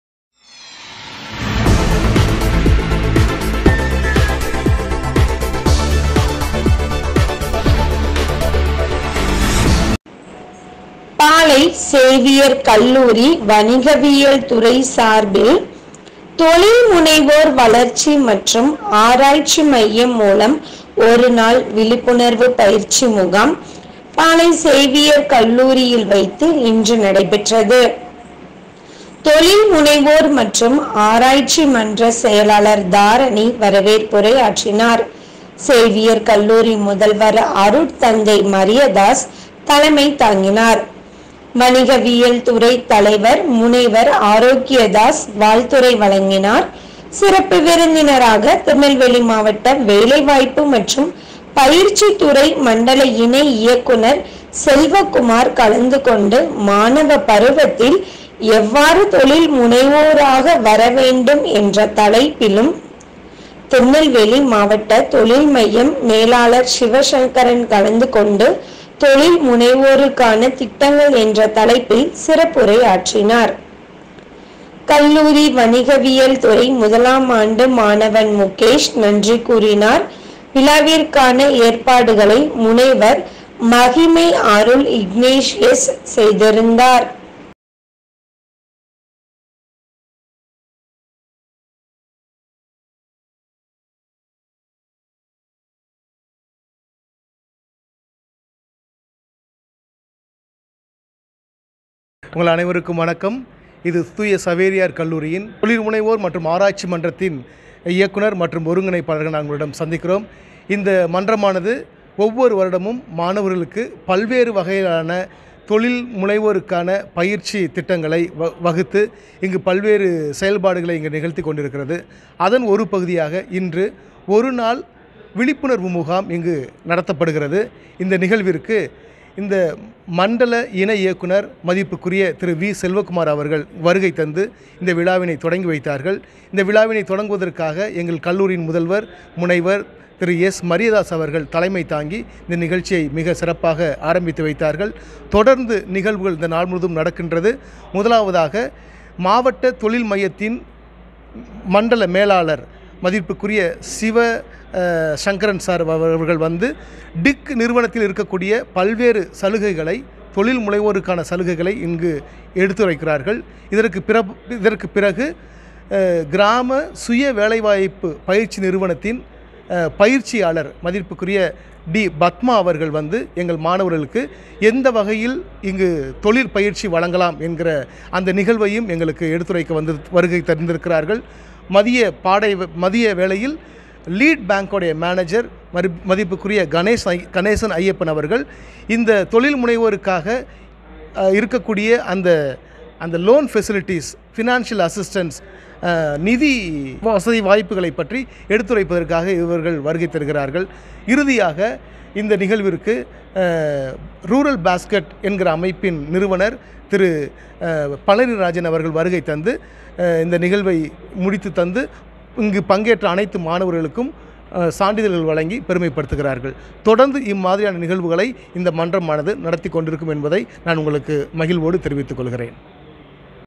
பாளை சேவியர் கல்லூரி வணிகவியல் துறை சார்பில் விழிப்புணர்வு பயிற்சி முகாம்.
பாளை சேவியர் கல்லூரி வணிகவியல் துறை சார்பில் தொழில் முனைவோர் வளர்ச்சி மற்றும் ஆராய்ச்சி மையம் மூலம் ஒரு நாள் விழிப்புணர்வு பயிற்சி முகாம் கல்லூரியில் வைத்து நடைபெற்றது.தொழில் முனைவோர் மற்றும் ஆராய்ச்சி மன்ற செயலாளர் தாரணி வரவேற்புரை ஆற்றினார் சேவியர் கல்லூரி முதல்வர் அருட்தந்தை மரியதாஸ் தலைமை தாங்கினார். வணிகவியல் துறை தலைவர் முனைவர் ஆரோக்கியதாஸ் வாழ்த்துரை வழங்கினார். சிறப்பு விருந்தினராக திருநெல்வேலி மாவட்ட வேலை வாய்ப்பு மற்றும் பயிற்சித்துறை மண்டல இணை இயக்குனர் செல்வகுமார் கலந்து கொண்டு மாணவப் பருவத்தில் எவ்வாறு தொழில் முனைவோராக வர வேண்டும் என்ற தலைப்பிலும்,திருநெல்வேலி மாவட்ட தொழில் மையம் மேலாளர் சிவசங்கரன் கலந்து கொண்டு தொழில் முனைவோருக்கான திட்டங்கள் என்ற தலைப்பில் சிறப்புரையாற்றினர். கல்லூரி வணிகவியல் துறை முதலாம் ஆண்டு மாணவன் முகேஷ் நன்றி கூறினார்.விழாவிற்கான ஏற்பாடுகளை முனைவர் மகிமை அருள் இக்னேசியஸ் செய்திருந்தார்.